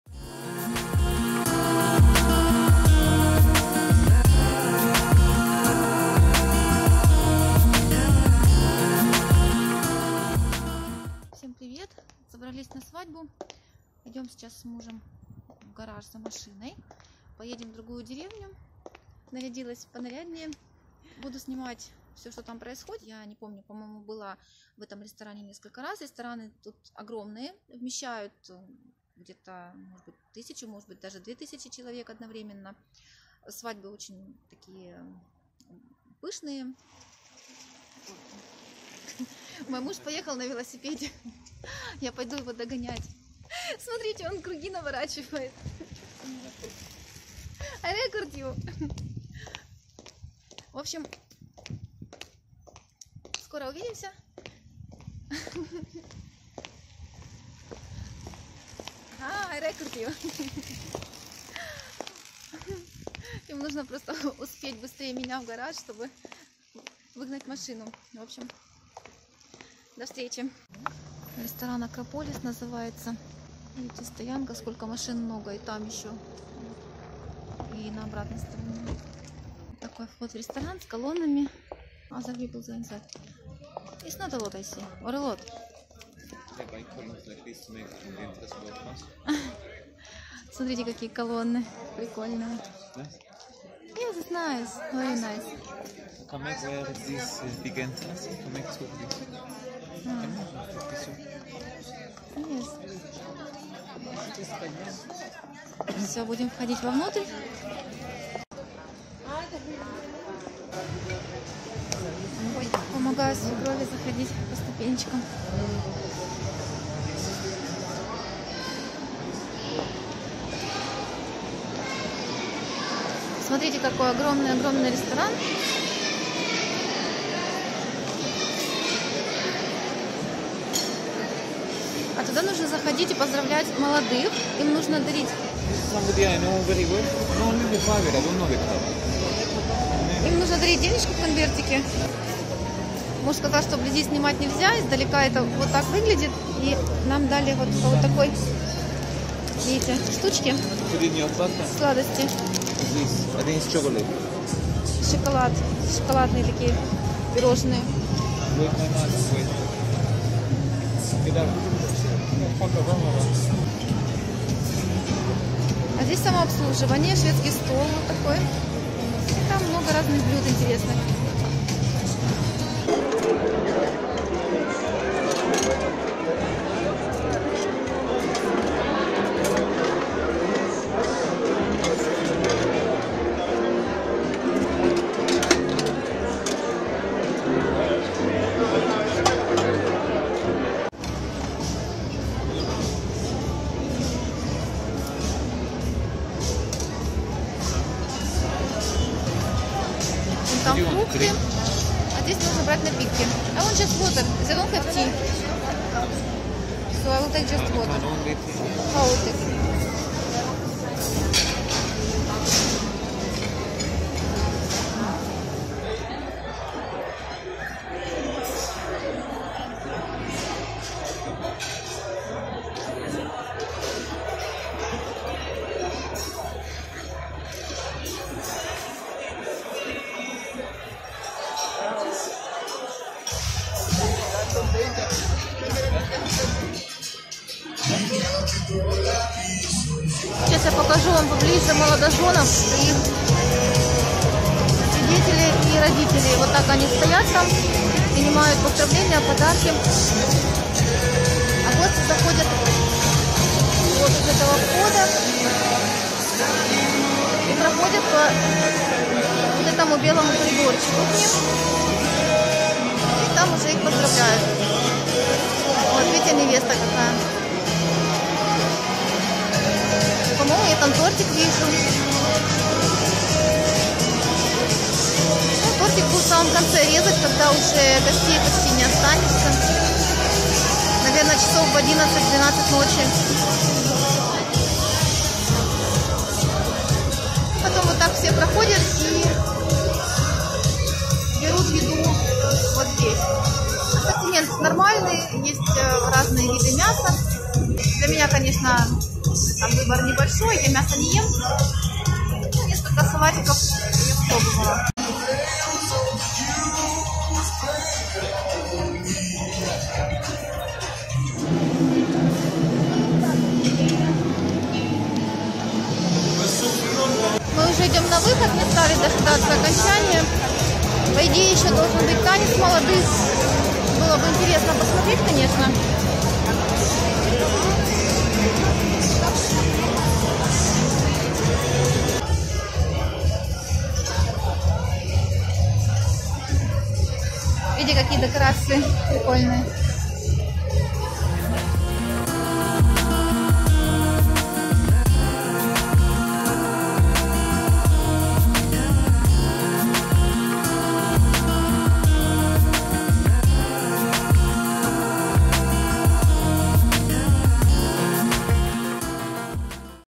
Свадьба у киприотов ♥ Ресторан Акрополис Палас, Кипр♥ Cyprus wedding
Всем привет! Сегодня делюсь с вами видео, снятым во время одного из посещений свадебного банкета. Все красиво, торжественно, достаточно спокойно (киприоты не пьют... ну почти все). Развлекательной программы обычно мало, бывает ведущий иди ДиДжей, может быть небольшое выступление с песнями, ближе к окончанию банкета - танец молодых и потом национальные хороводы и пляски с участием гостей. Для нас это слишком поздно происходит, мы редко досиживаем даже до раздачи свадебного торта. Но если как-нибудь удастся мне это заснять - сделаю еще одно видео!